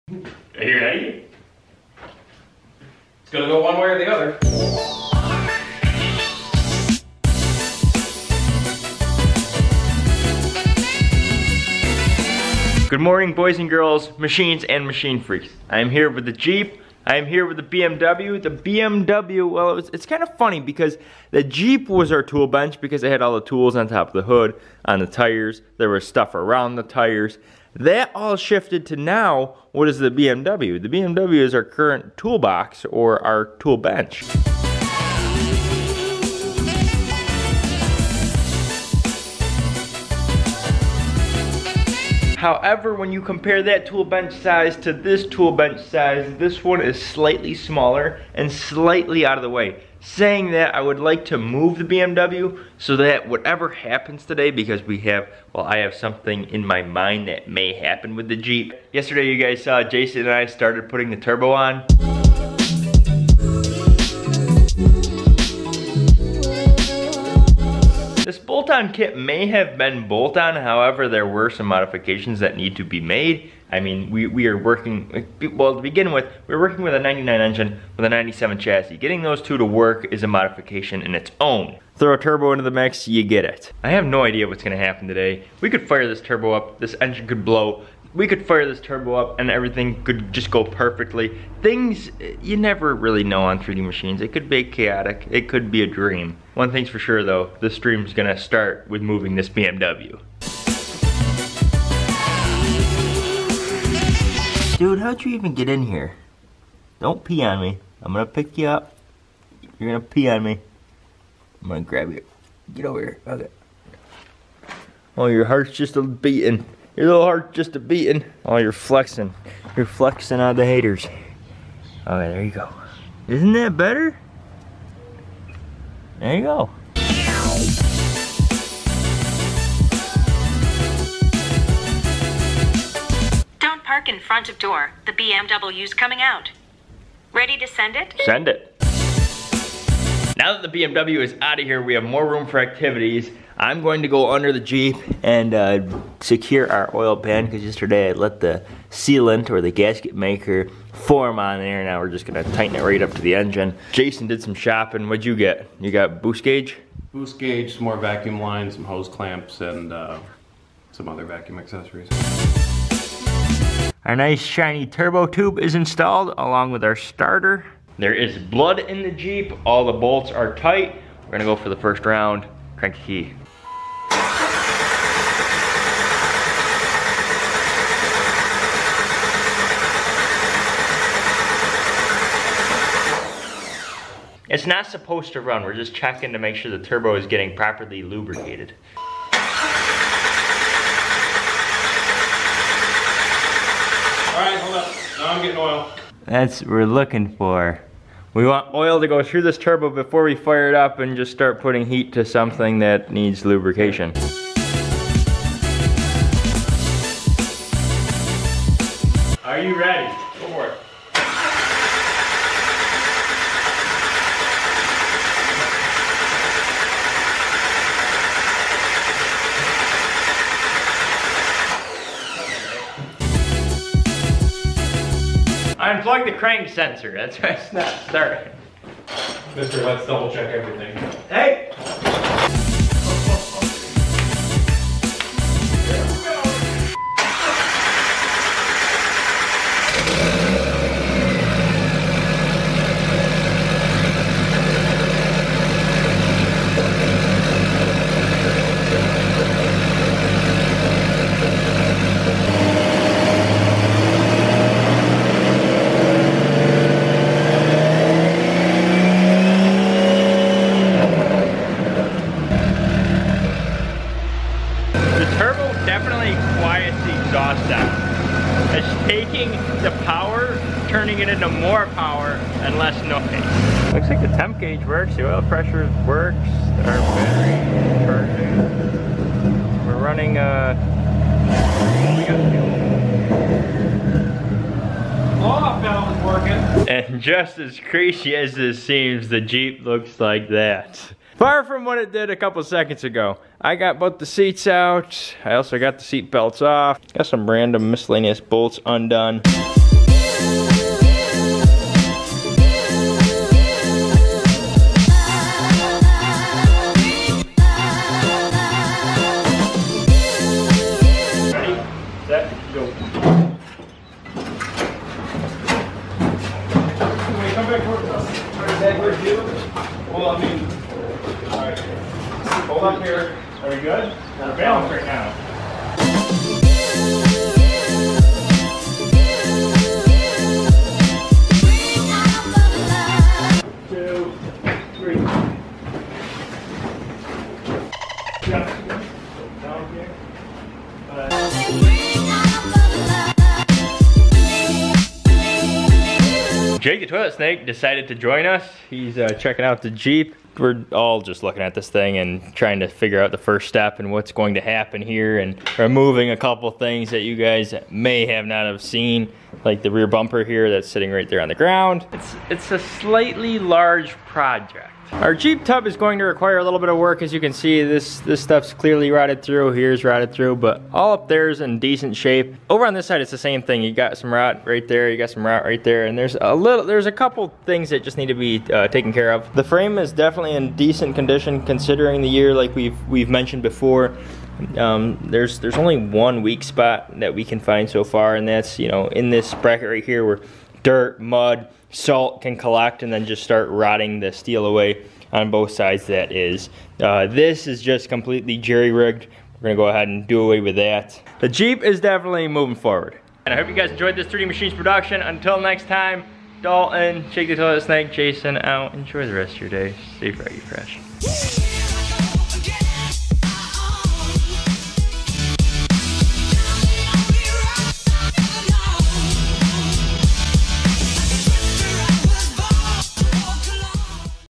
So Much Turbo Spool! Turbocharged Jeep
The turbo on this Jeep spools so much it's scary. LOL!

FAN MAIL:
3DMachines